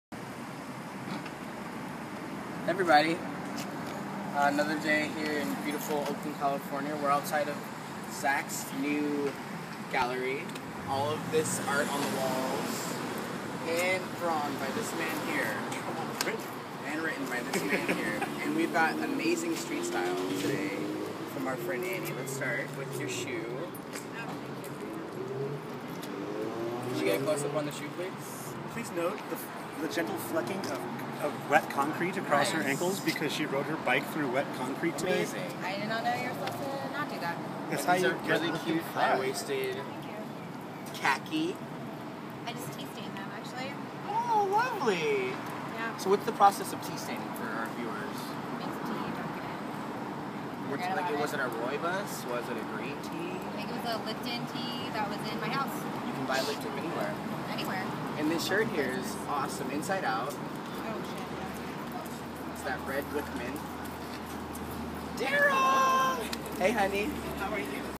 HAH STYLE: Oakland street style fashion and art najee rene interviews
Najeerene.bandcamp.com Najee Rene interviews zack Houston and Annie at infinty art space, oakland ca.